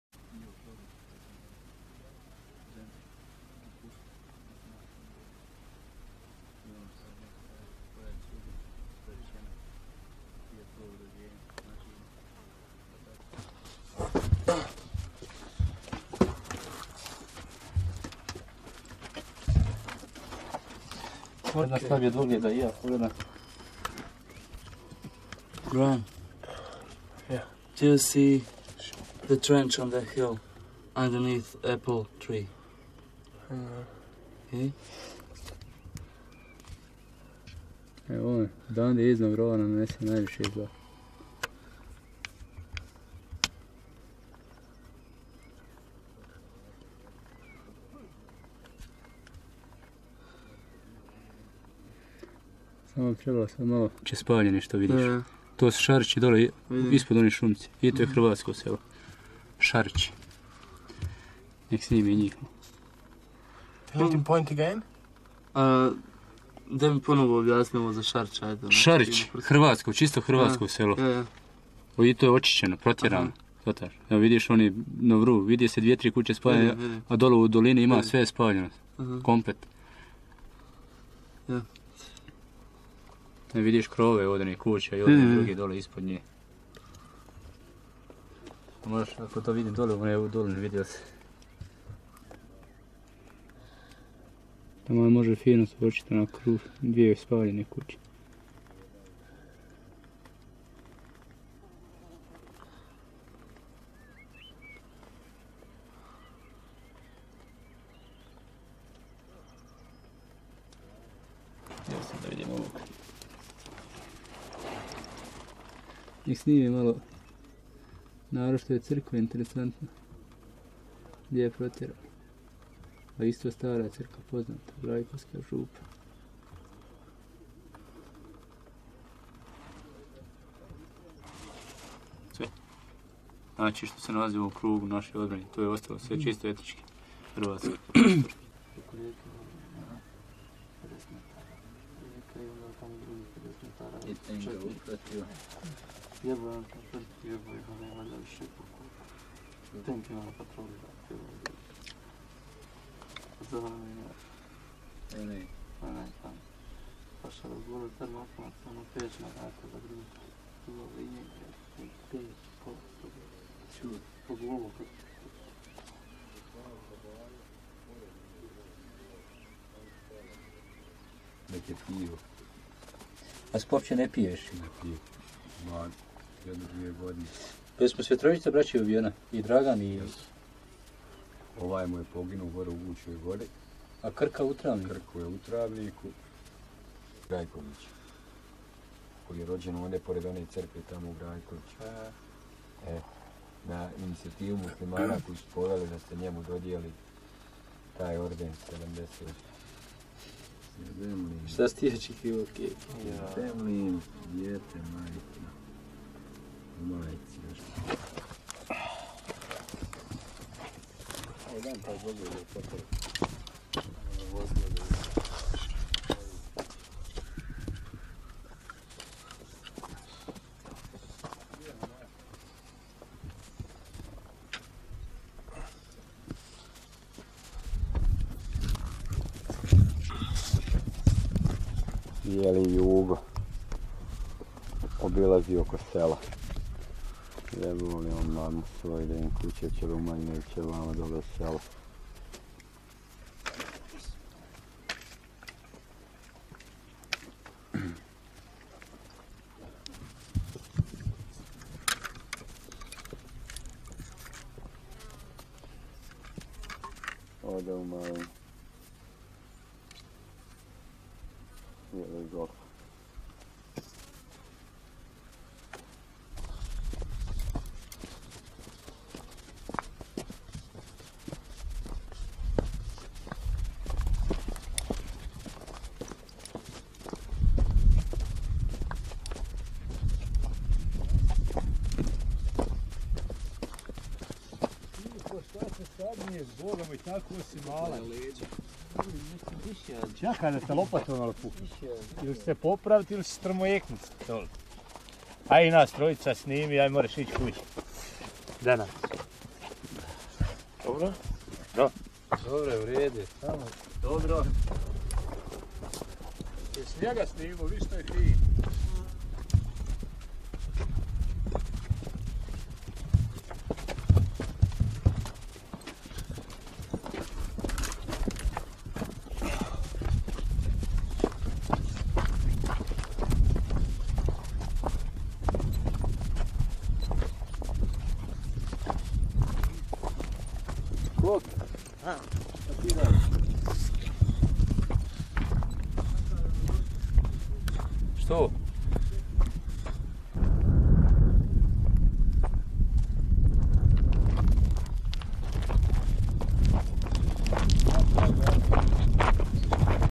Guča Gora - Lašvanska dolina 1993. (BiH) prva linija i rovovi HVO-a i hrvatske kuće u plamenu
Guča Gora - Lašvanska dolina 1993. Prva linija i rovovi HVO-a i hrvatske kuće u plamenu